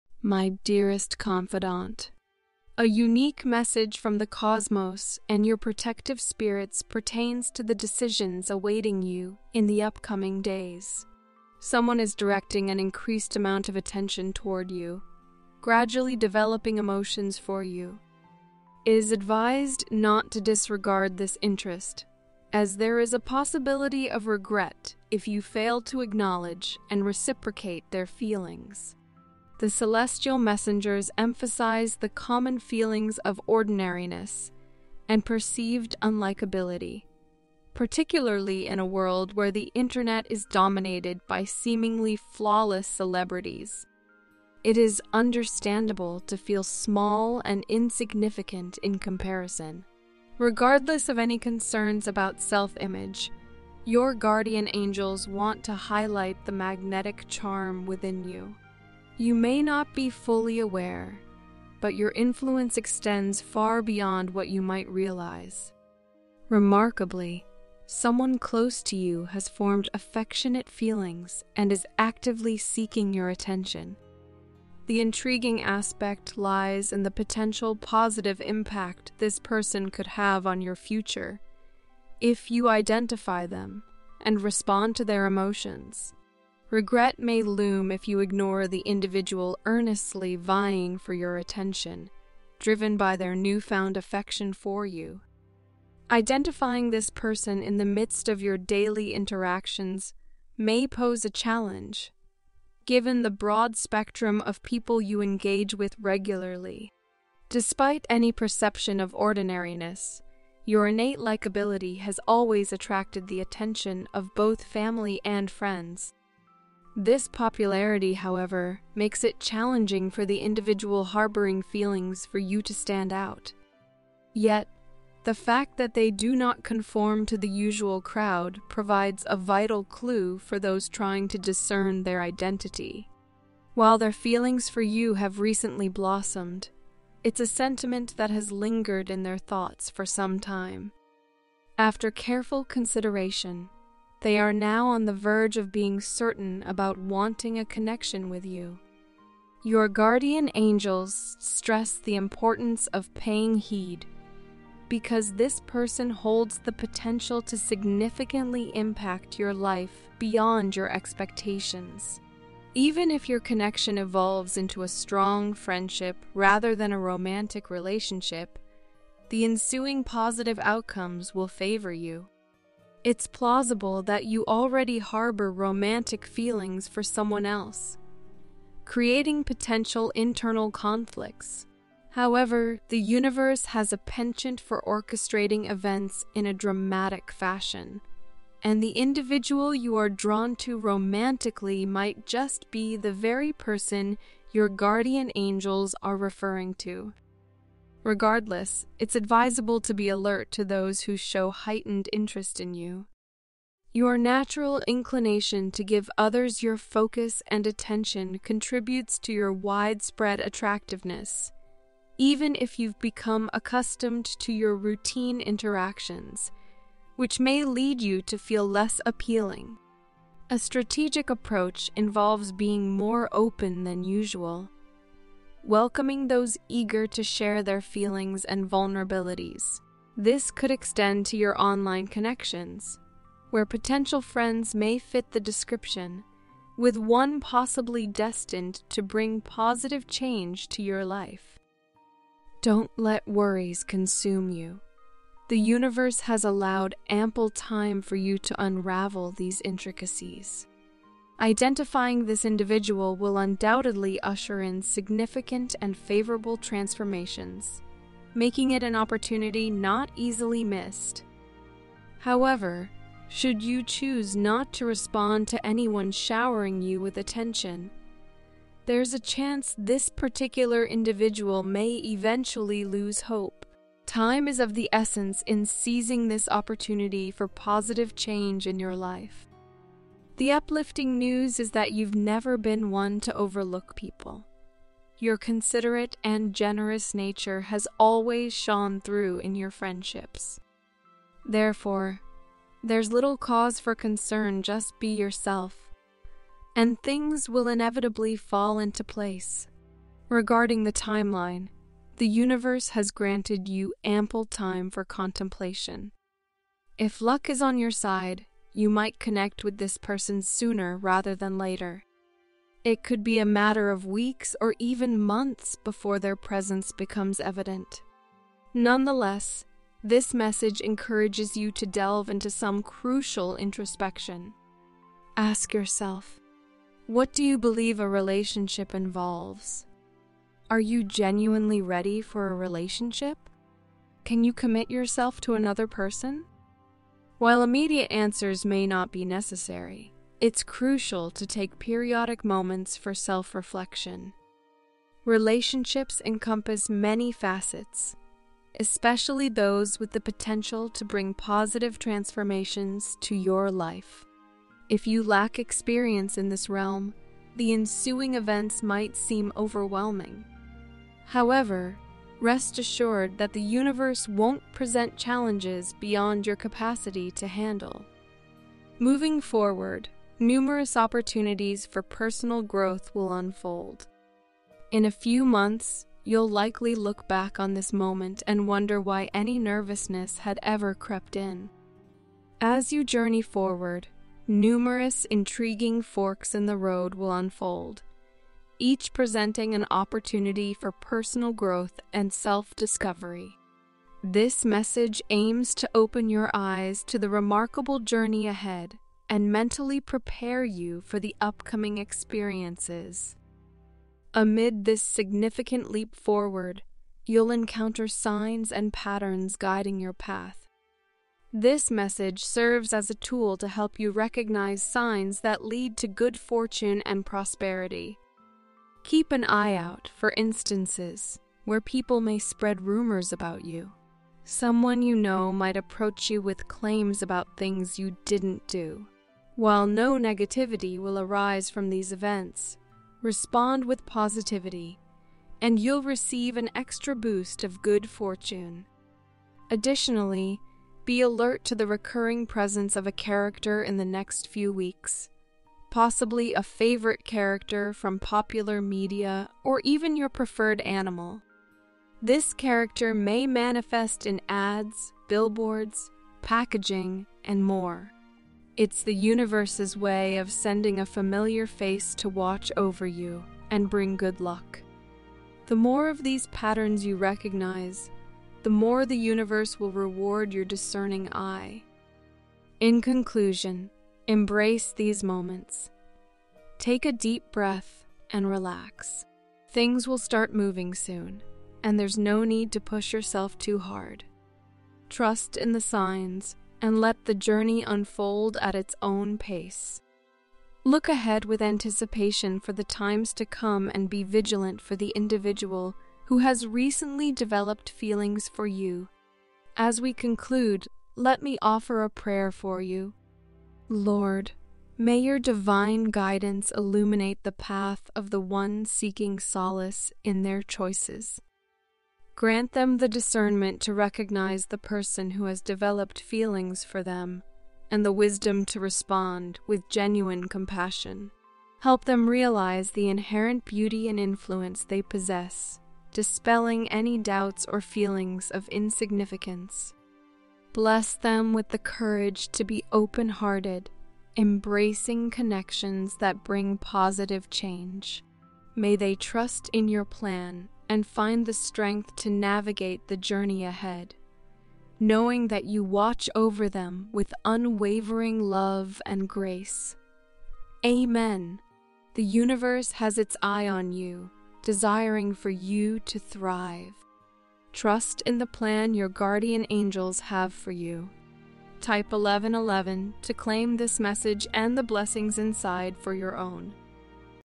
💌Your person is with other women they believe that ...
💌Your person is with other women they believe that😭channeled messages from your person